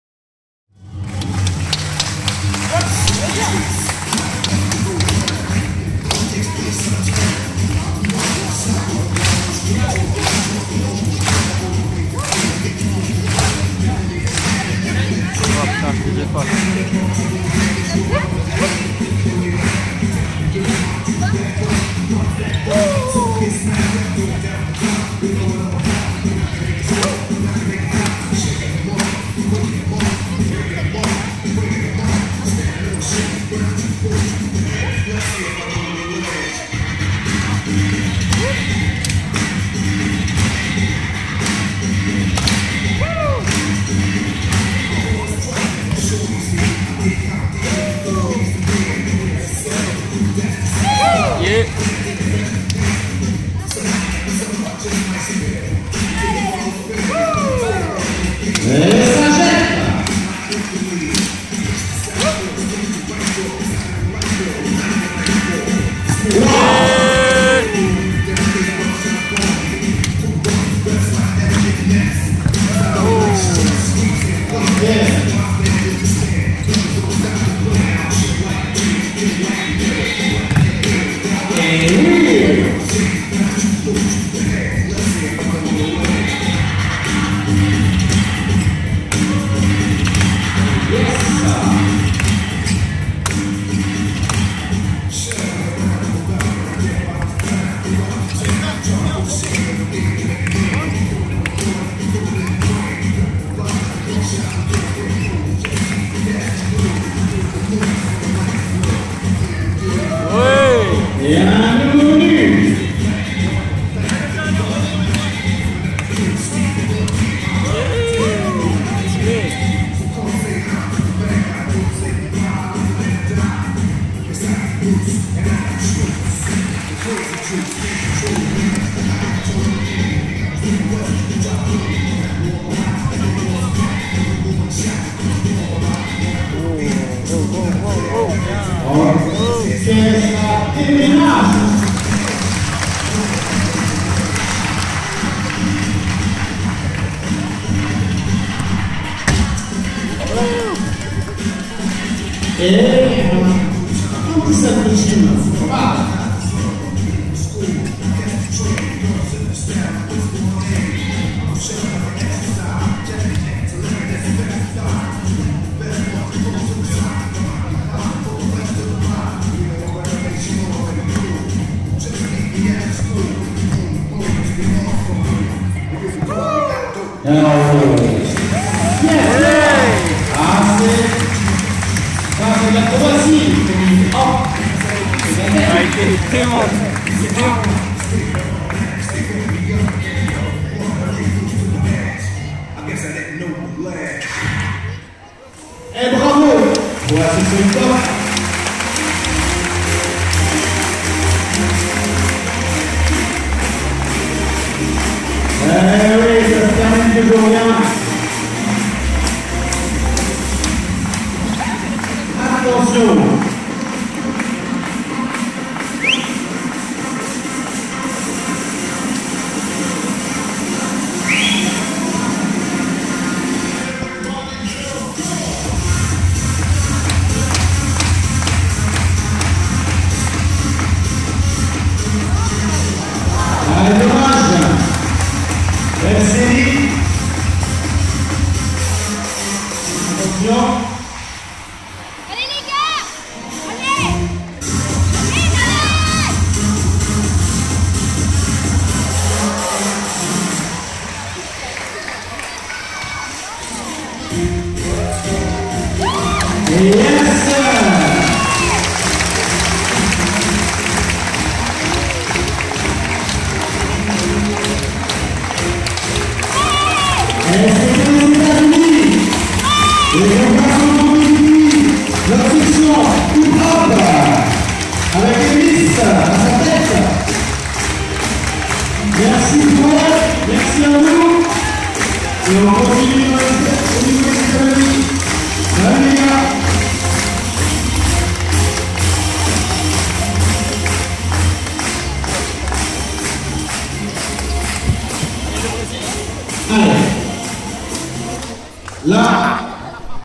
Vauzelles 2015 / In6dance Crew
Spectacle pour le Gala de gym des Vauzelles basketteur vs breakdancer